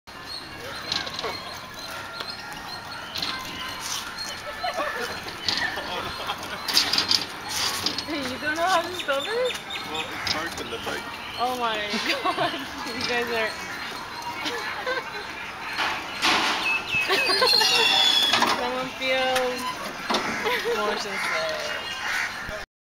Twinspin Ride
We ran across this at the VGP Universal park. Only one problem... there was no way to stop it.